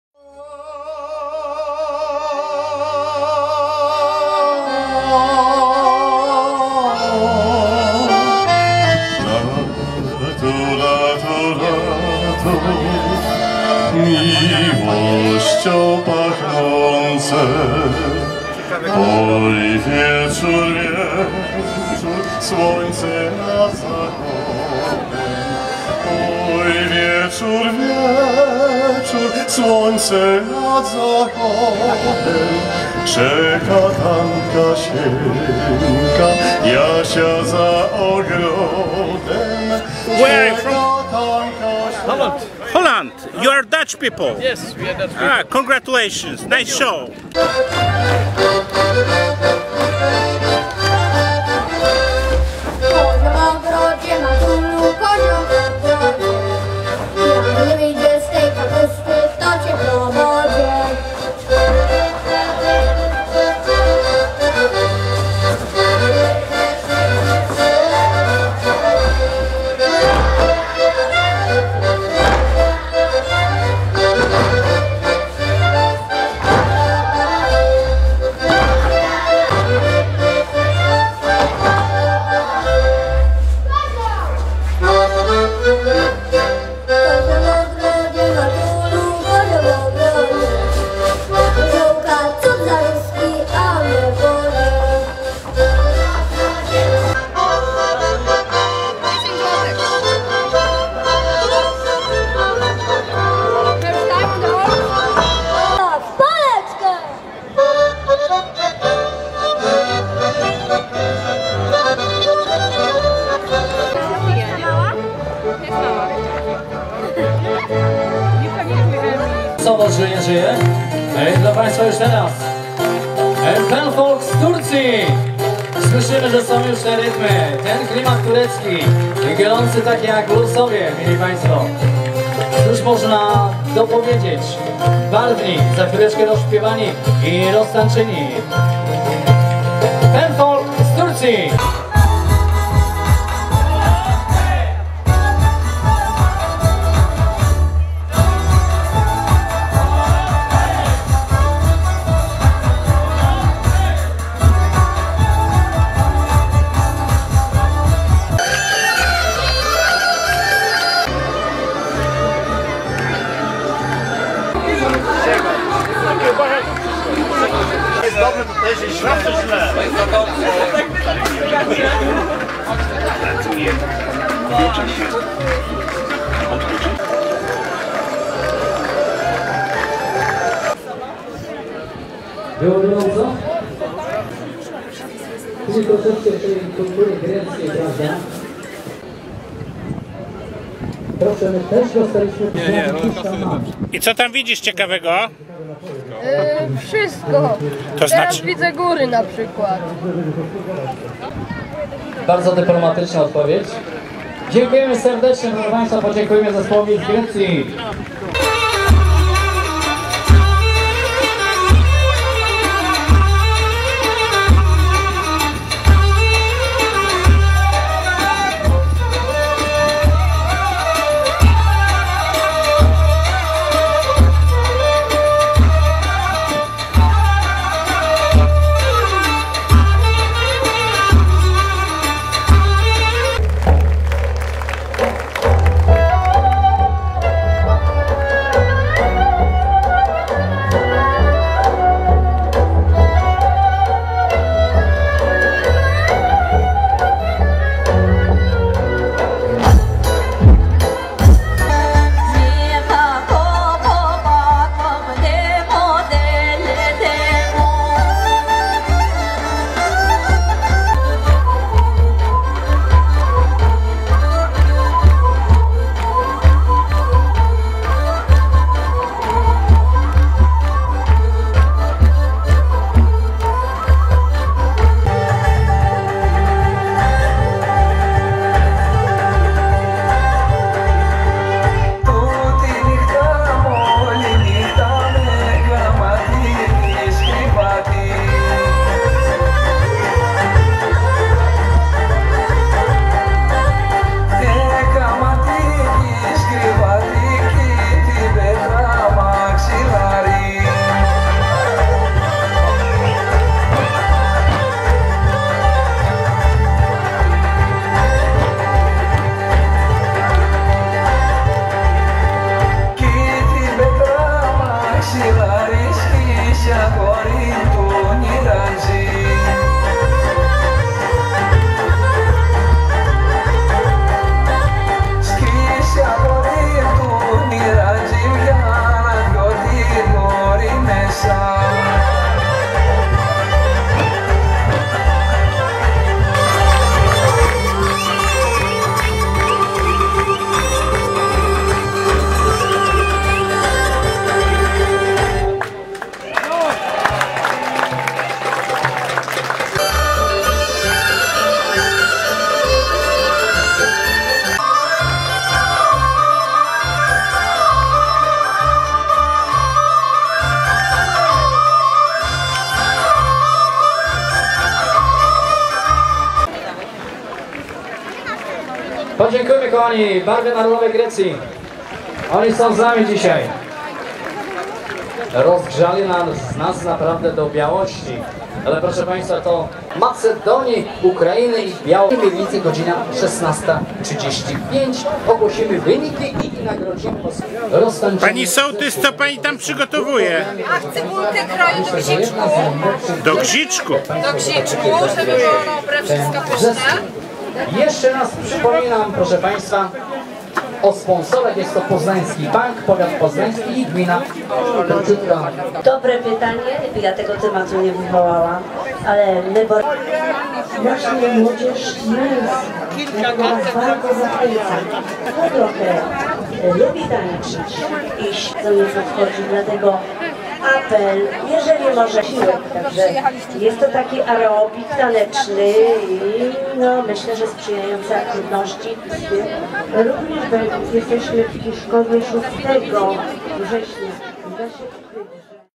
Gminne dożynki w Lusowie
Barwny reportaż z gminnych dożynek (Tarnowo Podgórne), które odbyły się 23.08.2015 we wsi Lusowo. Wydarzenie było połączone z międzynarodowym festiwalem folklorystycznym.